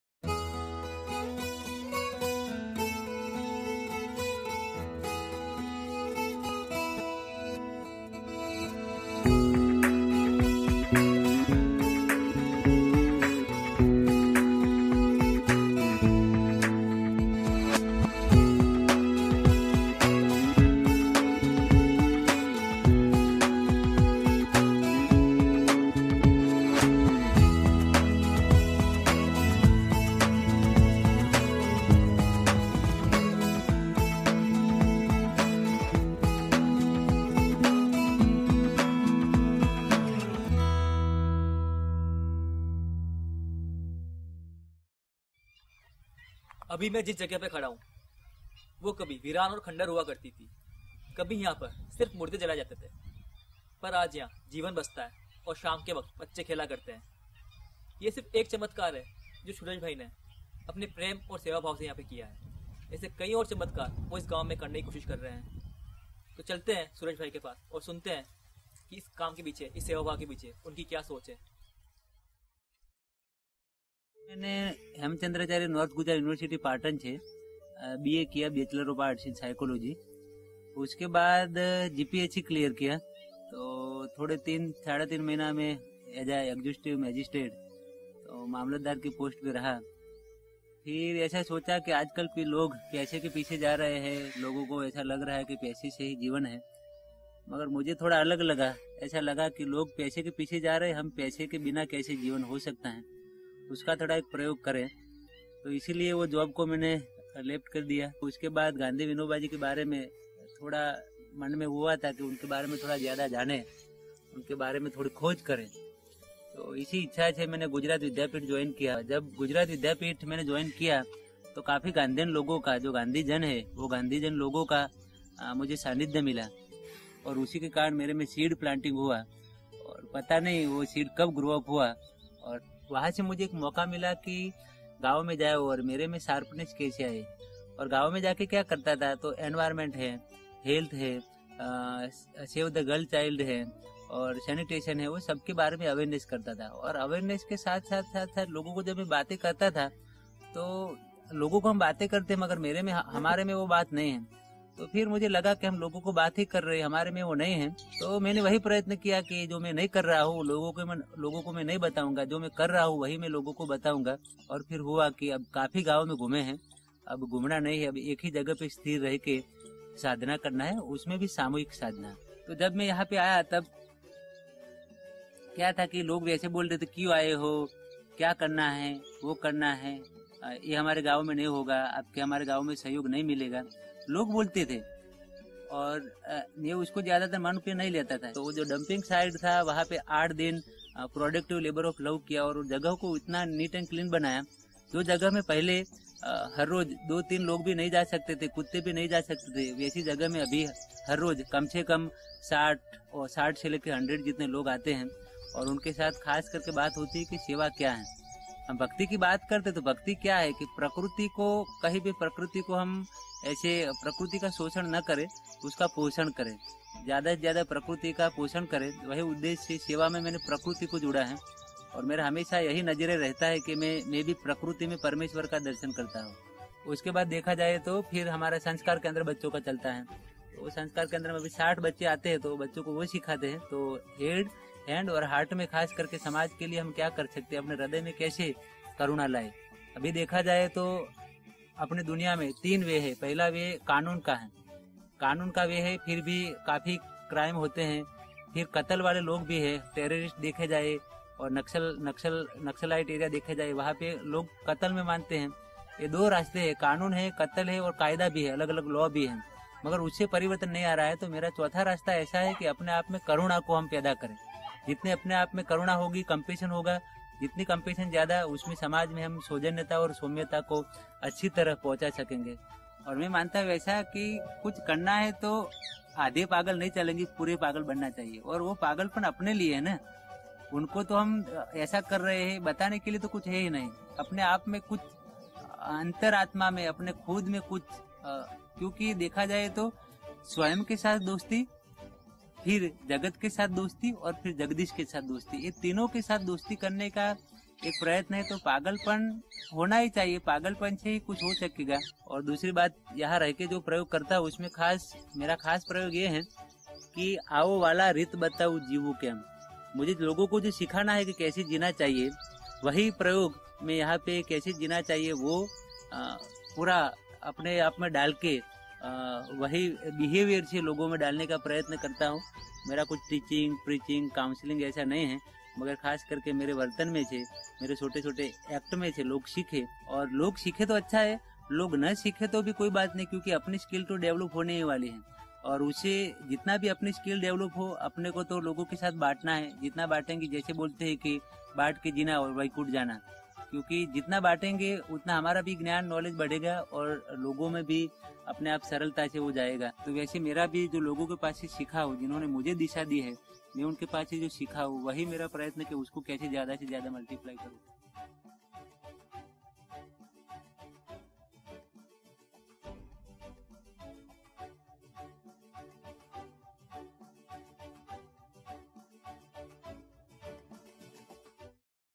# 8 Creating a bond with environment : 52 Parindey
Suresh Bhai set up an alternate education centre or a school of fun in Wakenada village, in Ahmedabad district of Gujarat, where he could nurture love, compassion and empathy among village children, thereby creating their bonds with environment. Besides this, Suresh has been advocating waste management and organic farming for the betterment of the society.